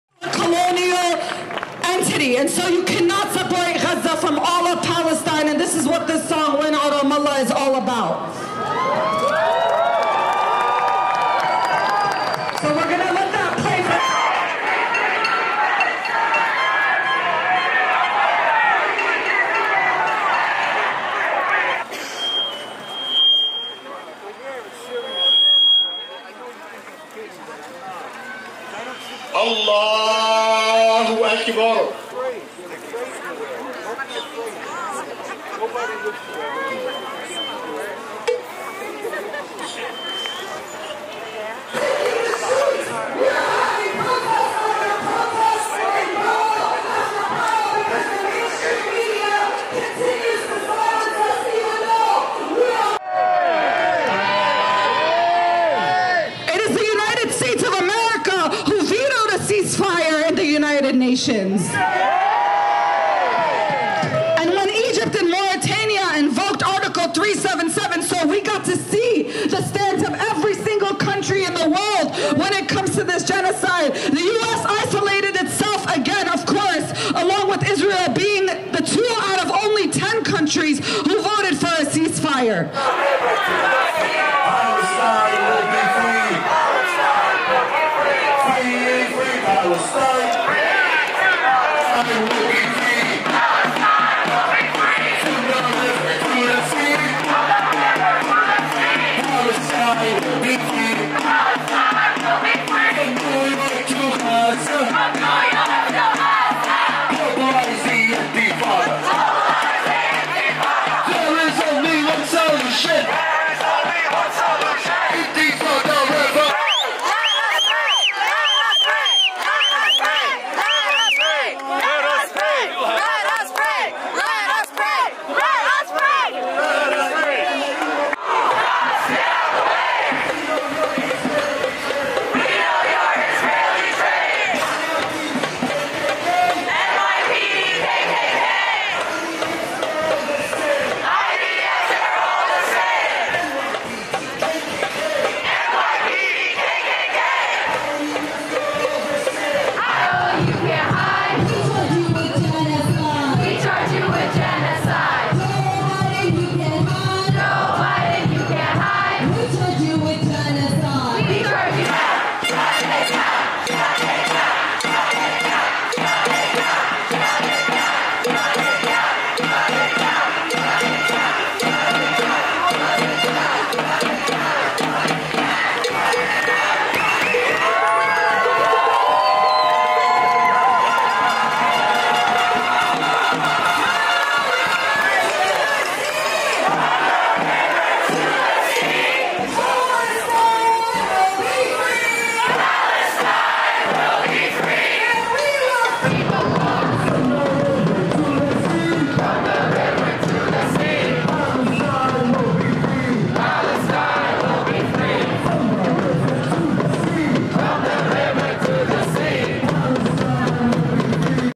Palestine Protesters Flood Astoria for Gaza
December 16 2023 QUEENS, New York